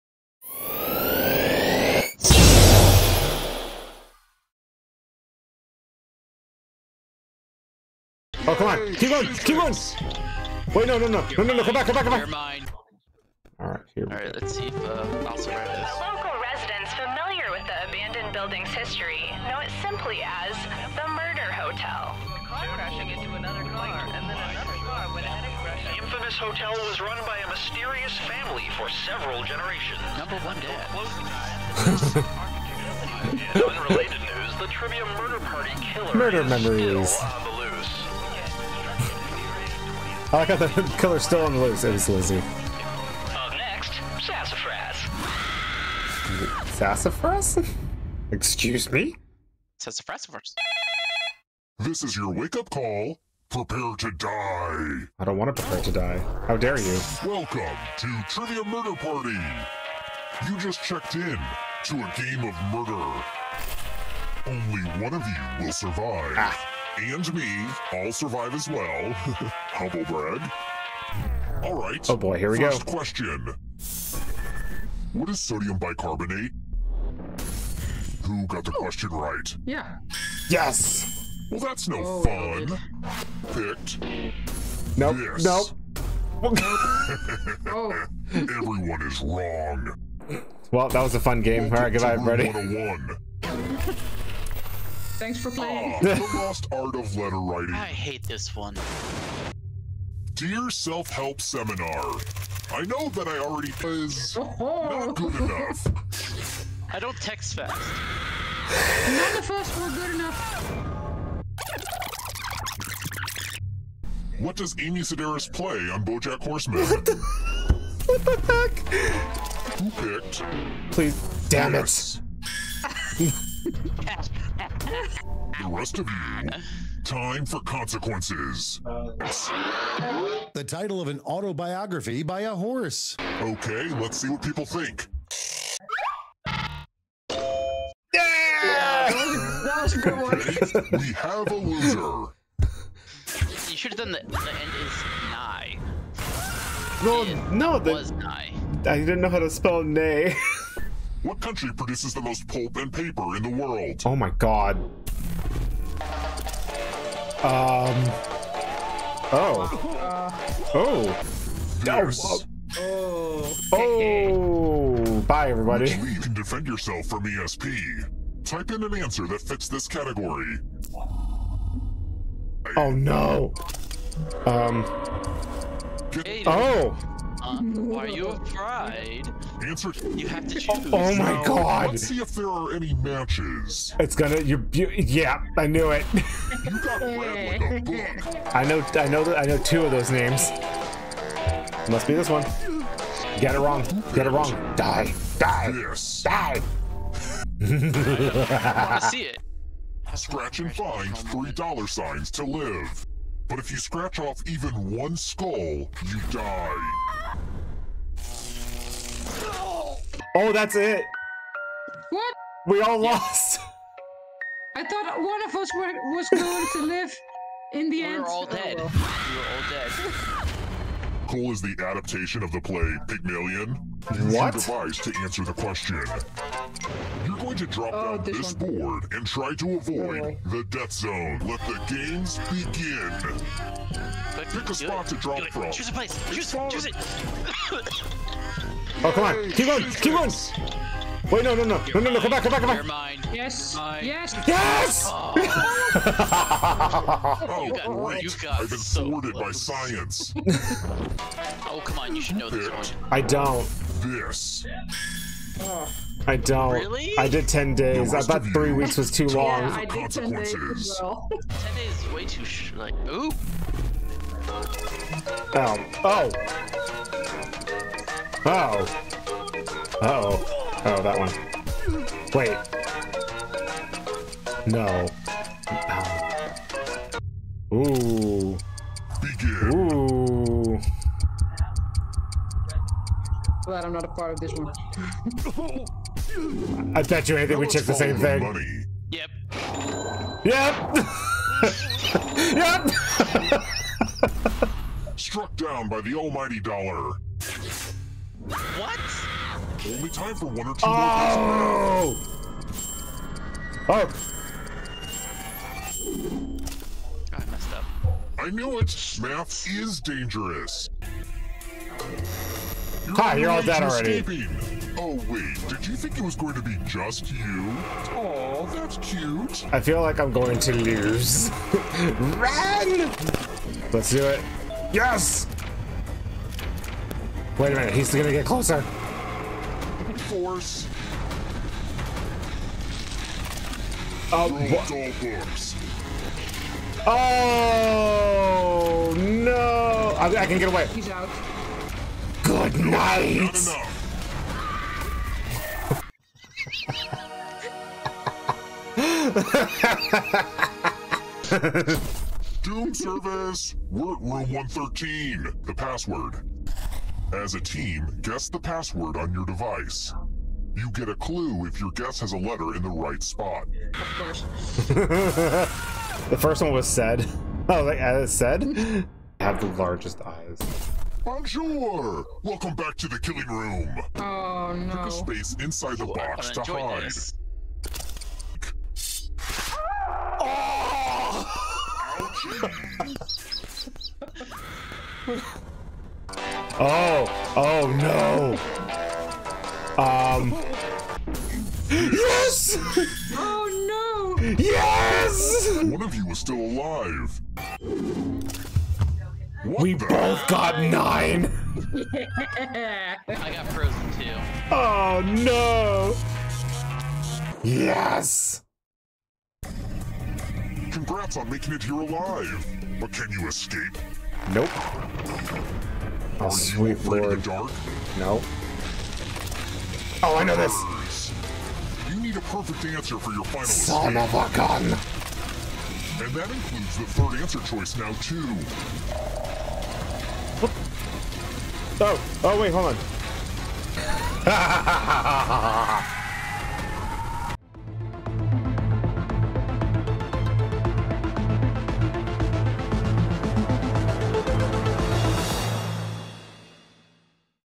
Sunday Gaming REcap Ep 4
Getting together with the team! Sunday Game Day!
The group gets together to play various games during the day and has a grand ol time.